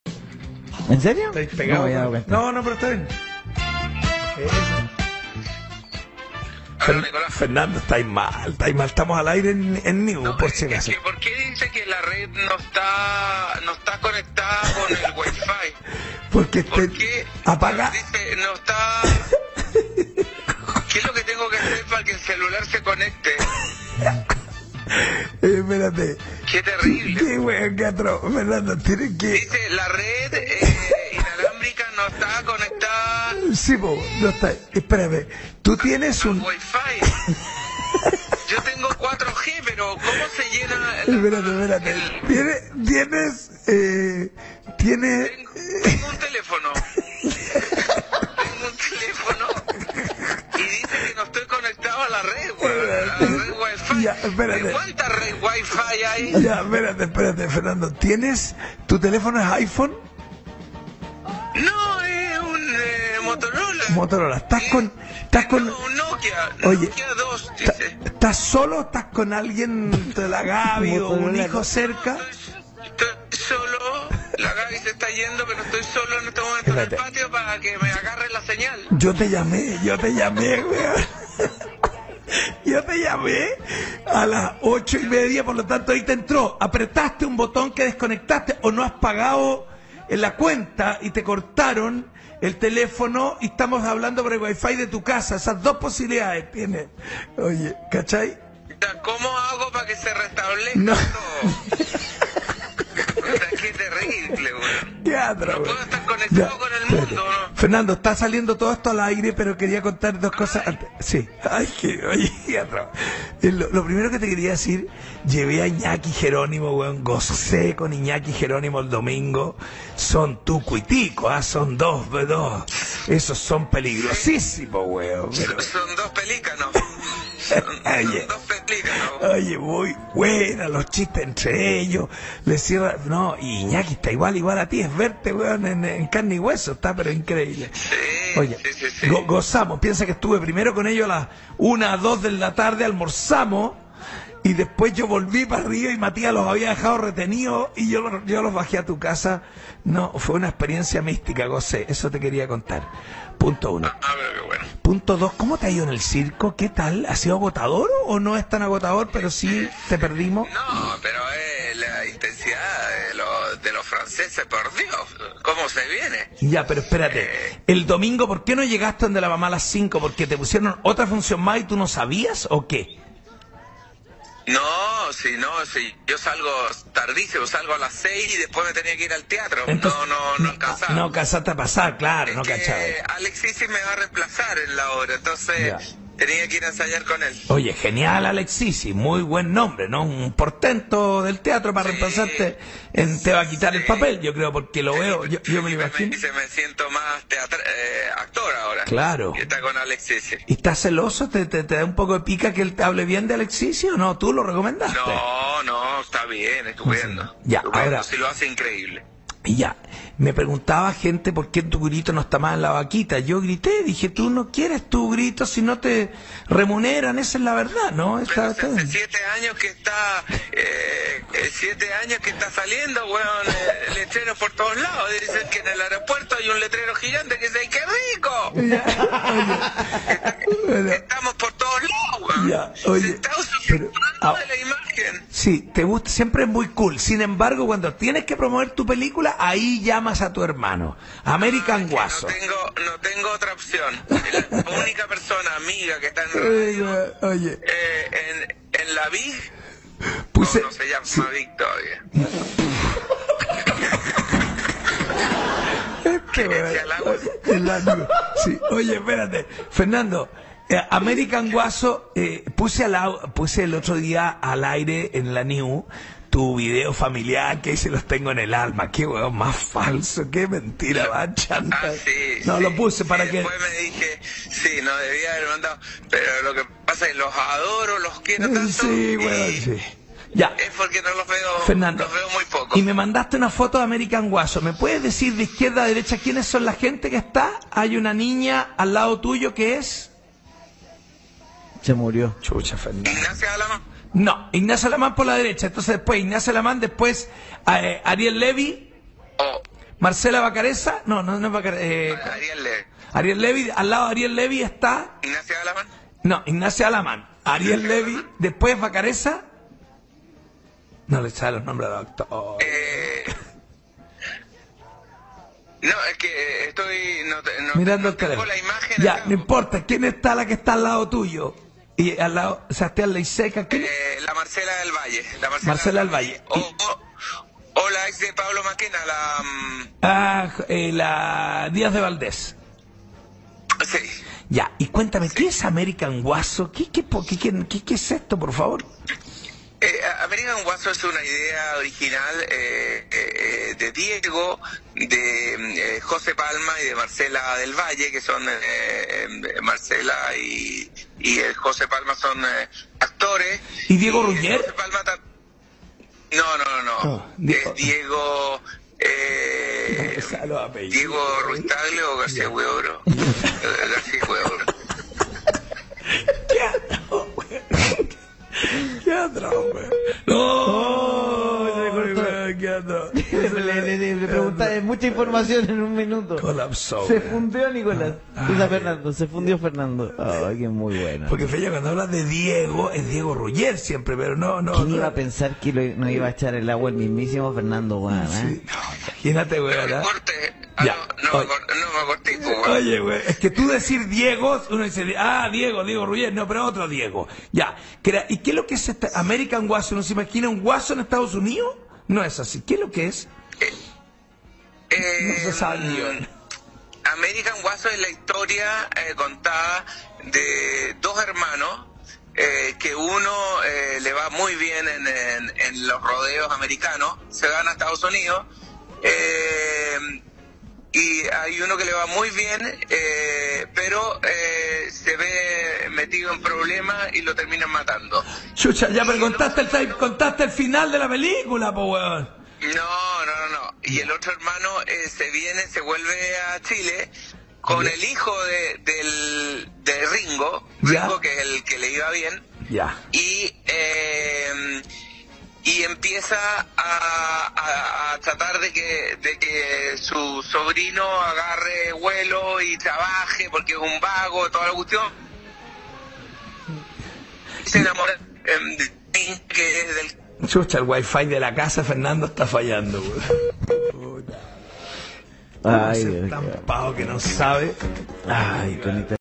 fernando larrain vs celular
nicolas ayudando a fernando con su celular desde la radio